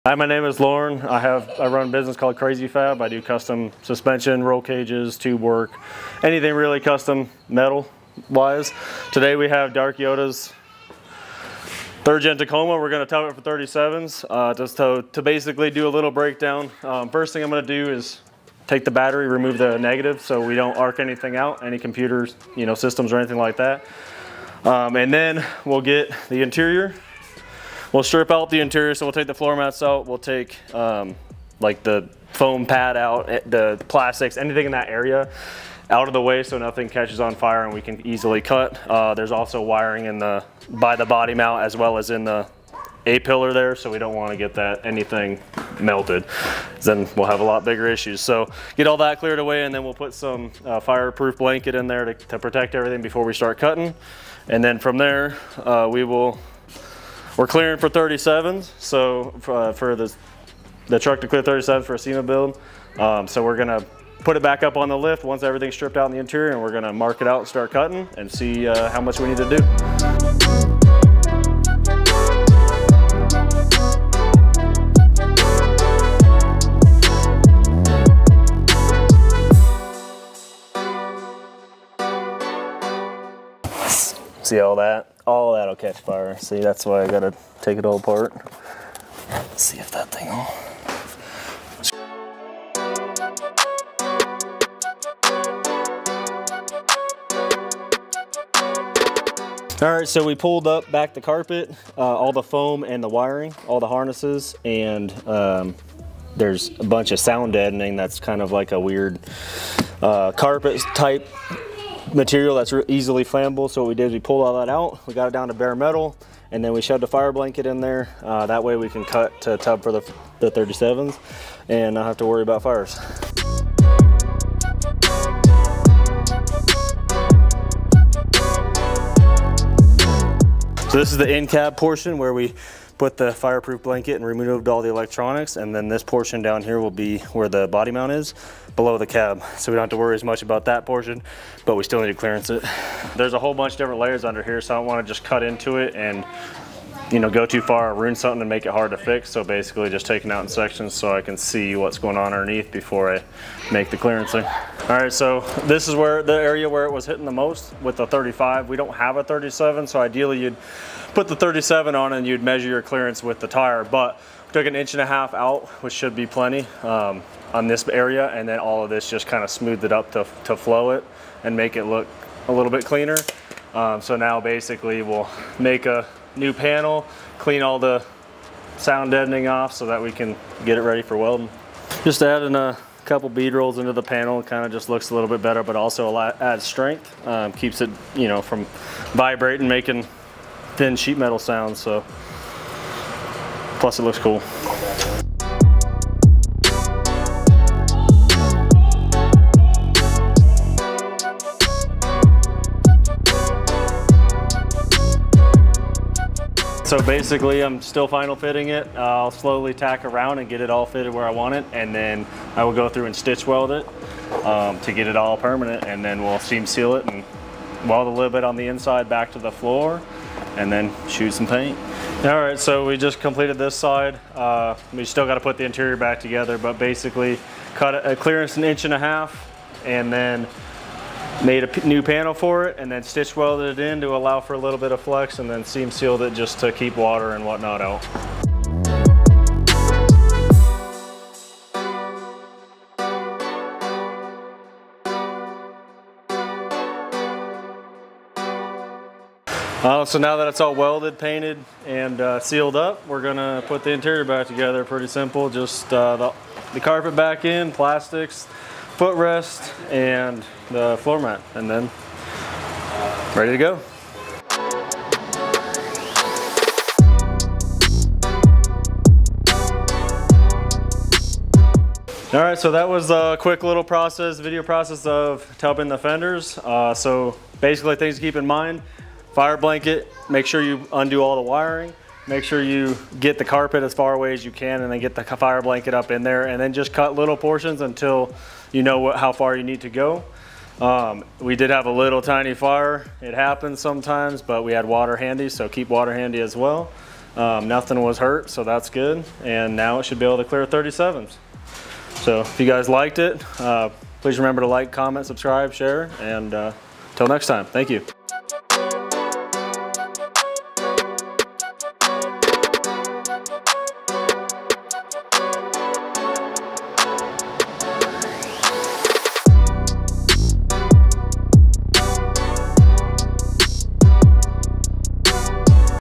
Tubing a 3rd gen Tacoma to fit 37s.
wheel well tubing on a 3rd gen toyota tacoma for @The_dark_yota to fit 37s.